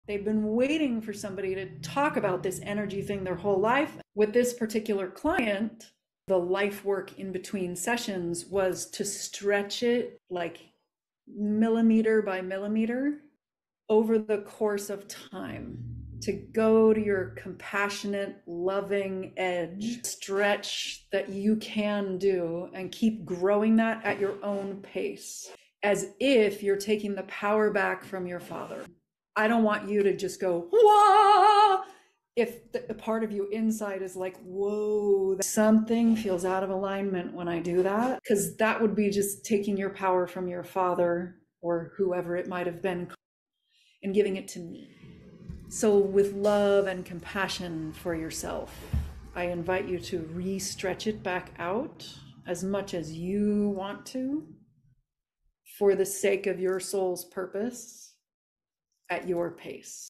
Take back your power, inch by inch ...
How to expand your energy and take your power back, one loving inch at a time.